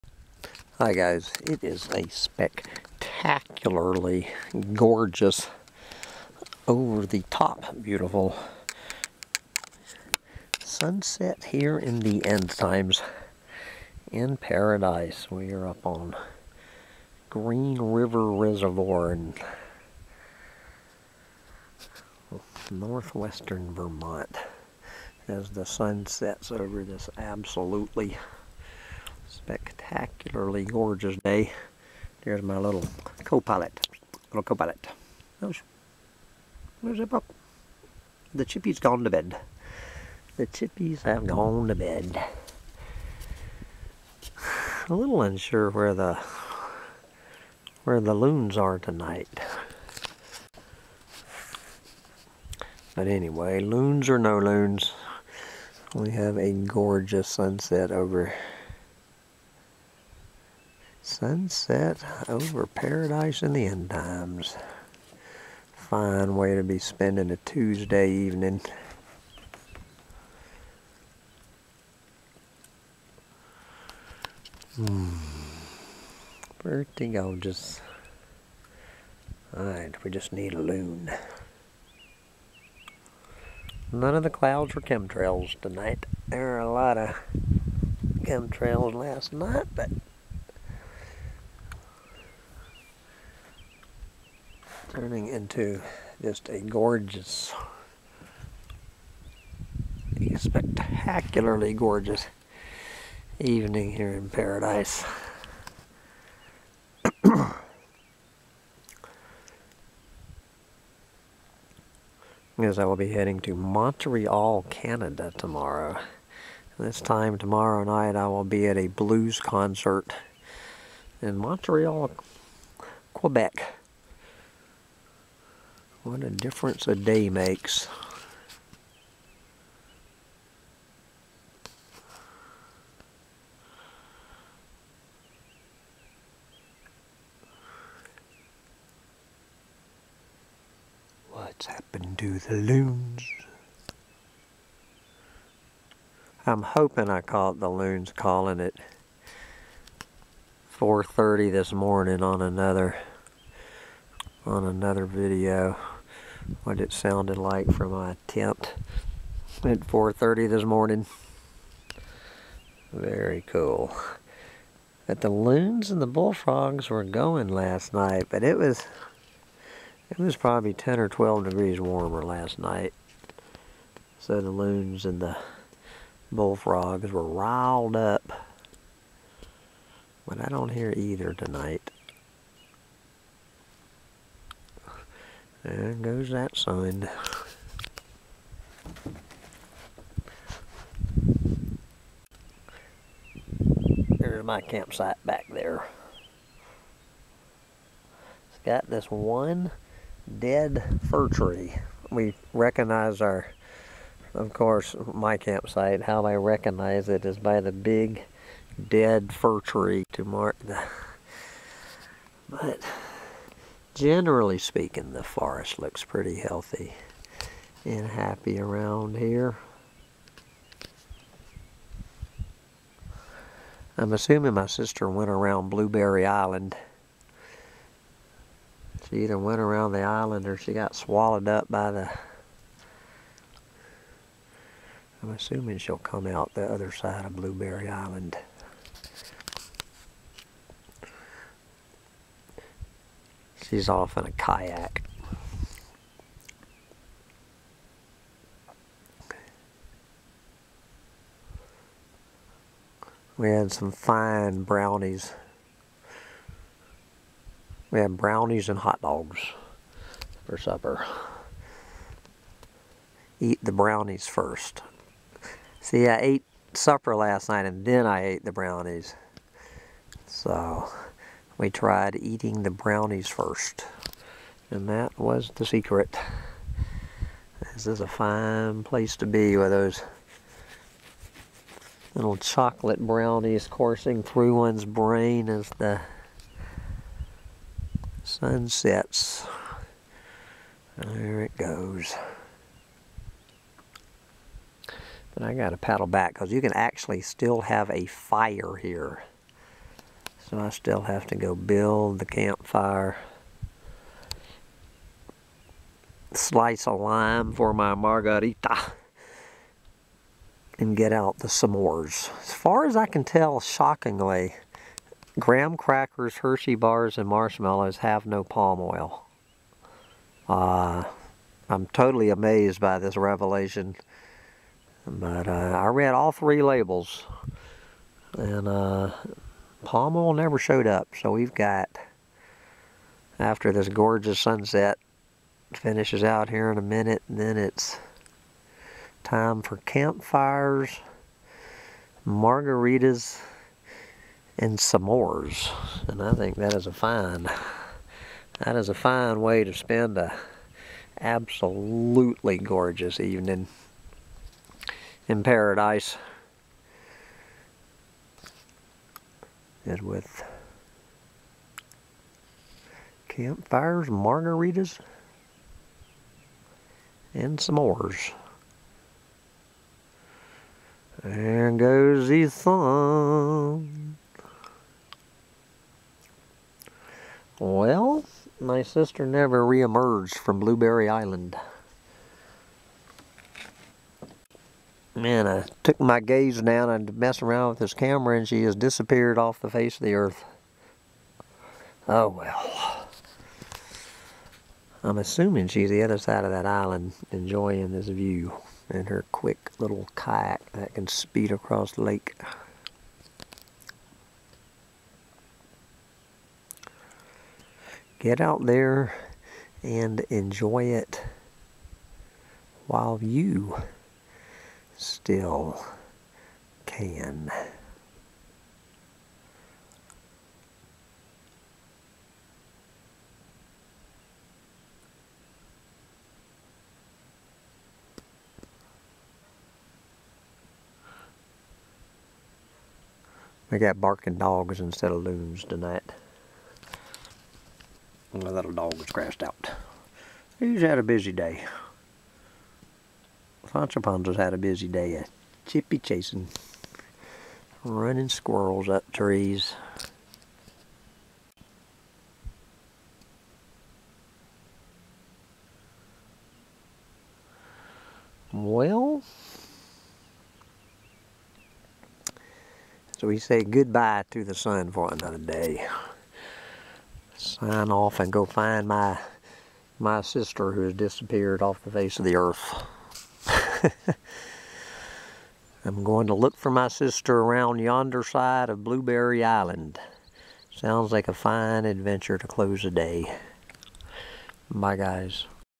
Sunset Over the End Times in Paradise in Vermont
It is a tough life and dirty job being a Doomsday Prophet and the chronicler of the End Times, but someone's gotta do it, so it may as well be me. Get out there and enjoy it while YOU still can.